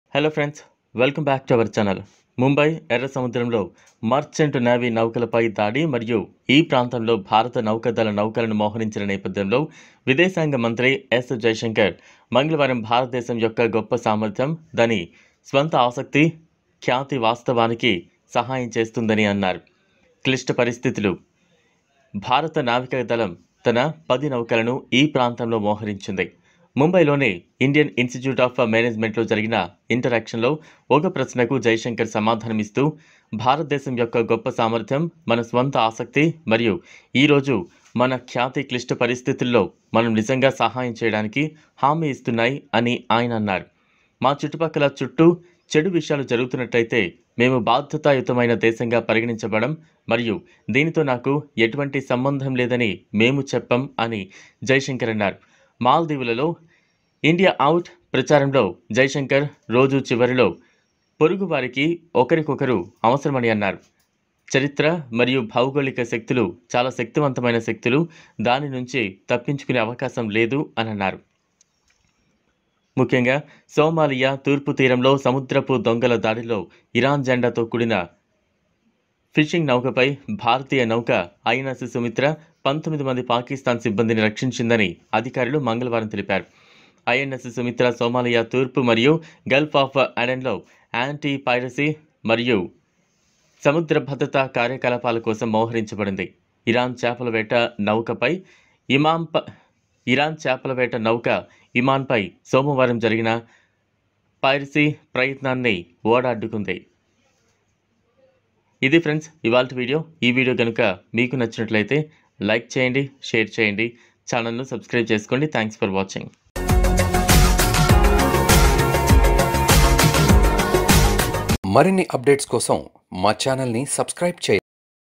ప్రాణాలకు తెగించి పాకిస్థాన్ ని రక్షించిన కమాండోలపై ఘోర వ్యాఖ్యలు చేసిన జై శంకర్.!| STAND FOR INDIA
WATCH;ప్రాణాలకు తెగించి పాకిస్థాన్ ని రక్షించిన కమాండోలపై ఘోర వ్యాఖ్యలు చేసిన జై శంకర్.!| STAND FOR INDIA